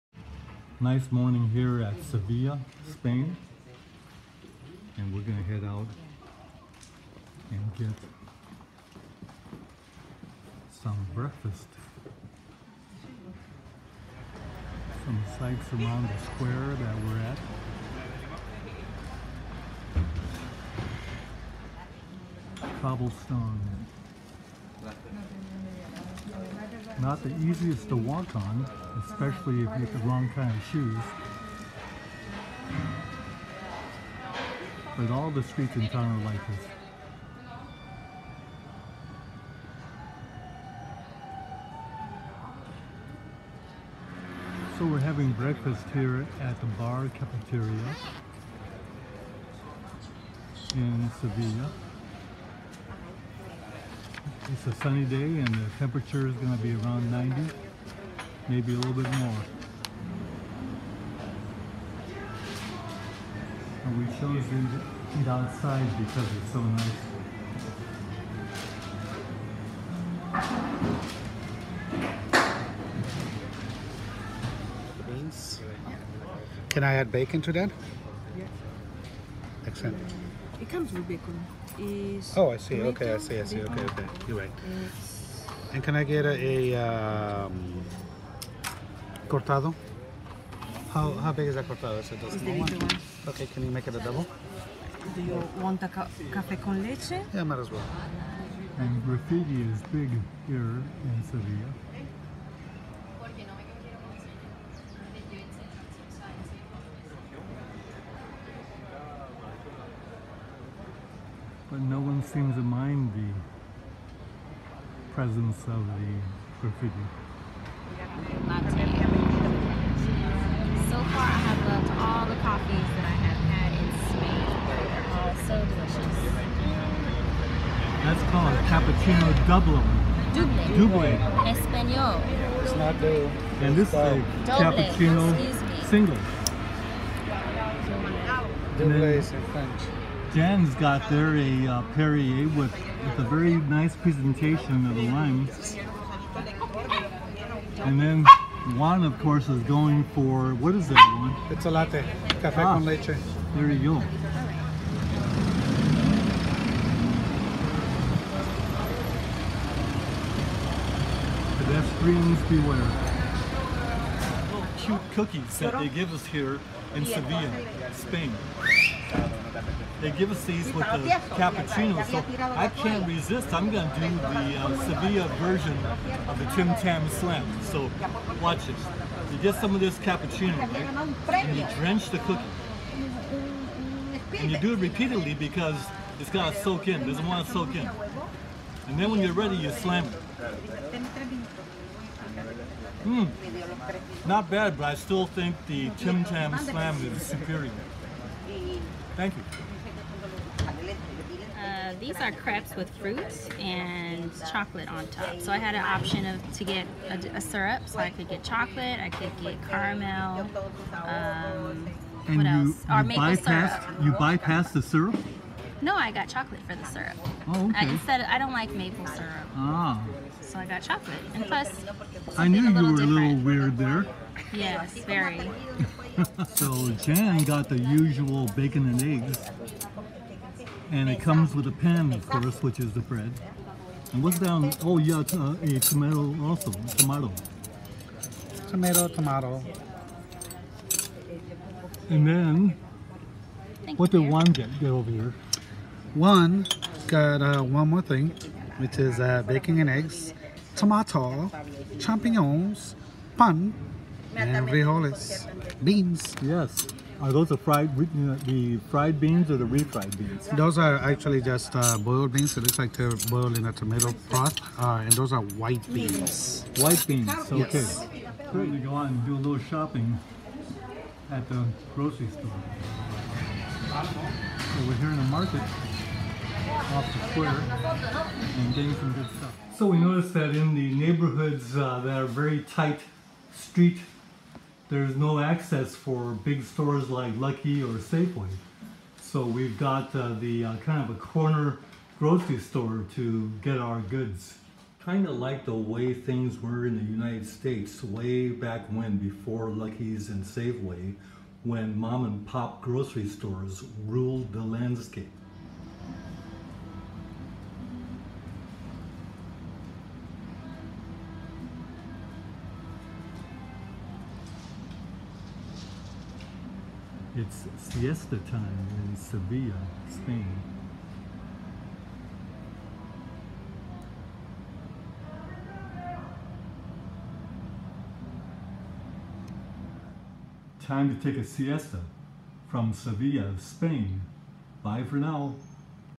Time In Southern Spain In Sevilla - Desayuno With Friends
Beautiful mornings in the Macarena District of Seville beckon breakfast with friends